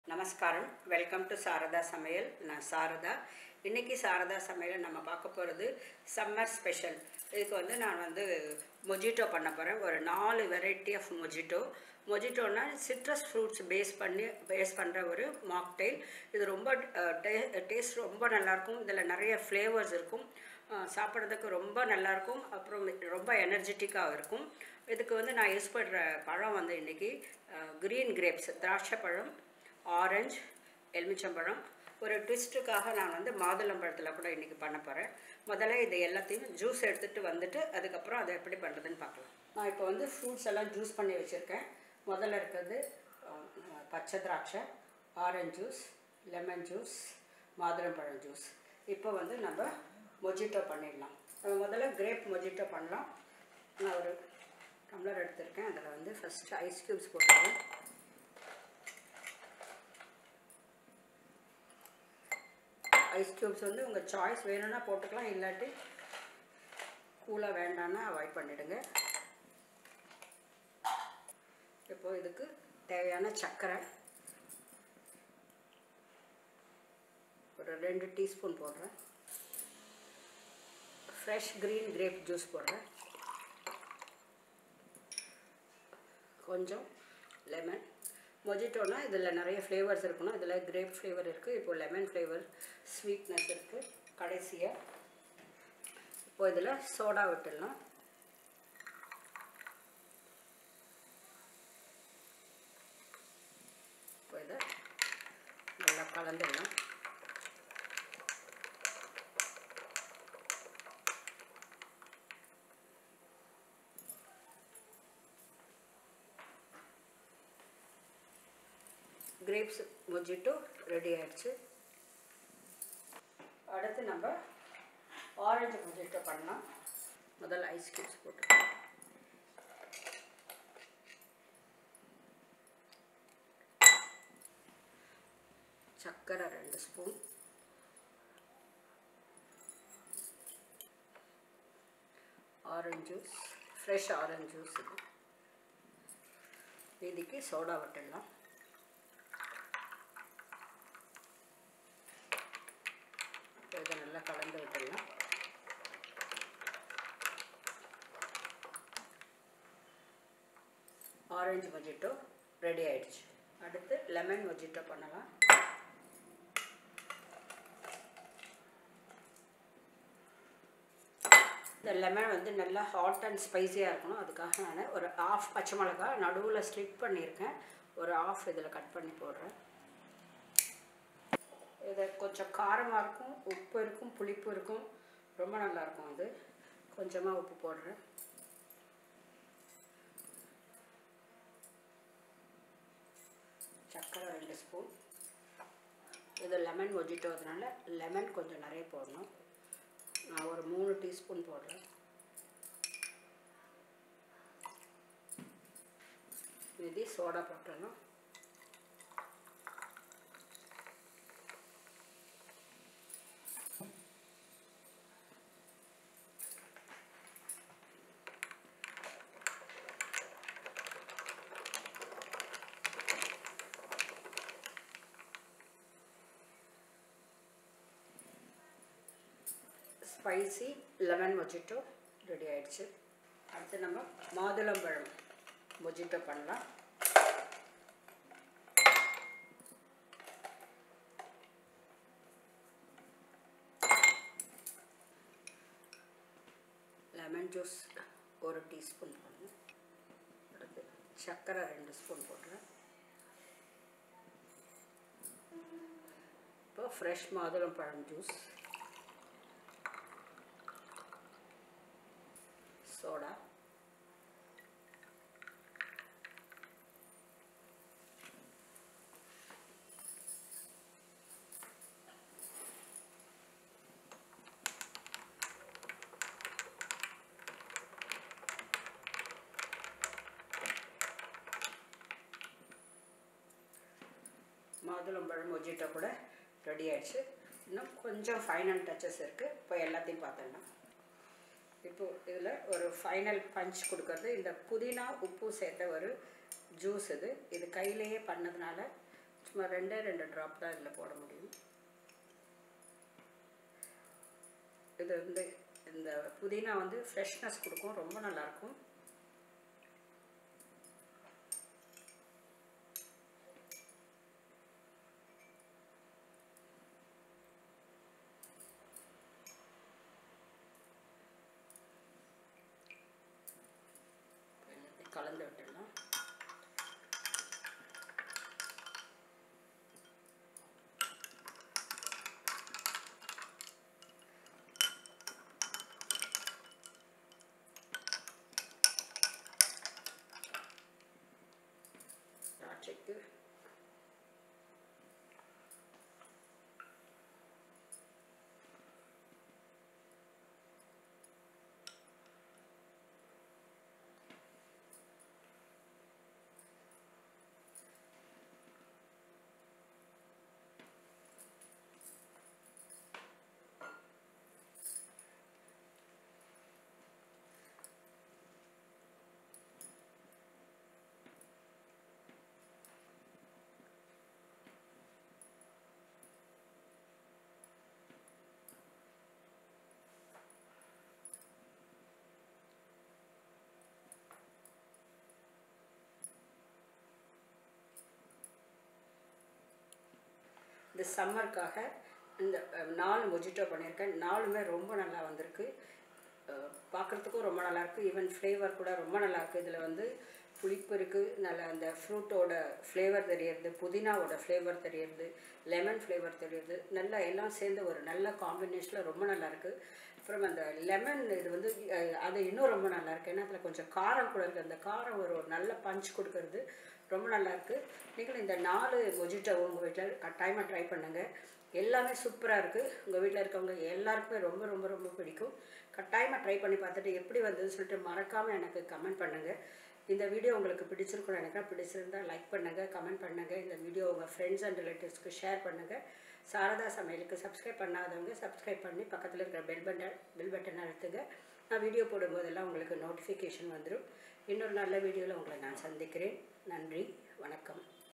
Four Flavours of Mojito| Spicy Lemon |Pomegranate |Grape & Orange Mojito
Mojito (/moʊˈhiːtoʊ/; Spanish: [moˈxito]) is a traditional Cuban punch. The cocktail   consists of    rum, sugar , lime juice, soda water &  mint. A pinch of salt & a slice of green chilly are also  added  for Lime Mojito. Combination of sweetness, citrus and   mint flavors make chilled    mojito a popular summer drink. s I made Mojito for my family, rum as been avoided.
in this Video, I have chosen four mojitos with  fresh juices  viz., pomegranate,  lemon, orange & grape.